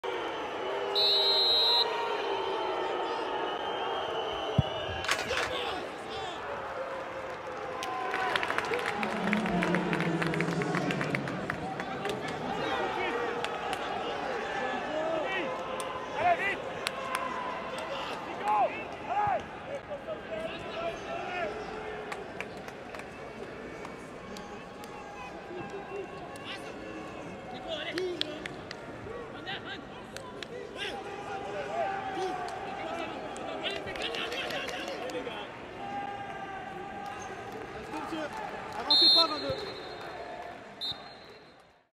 #06 : le but d'Imran Louza face à l'OGC Nice
Revivez, sous tous les angles, le but inscrit par Imran Louza lors de la défaite du FC Nantes contre l'OGC Nice (2-1) lors de la 6ème journée de Ligue 1 Uber Eats.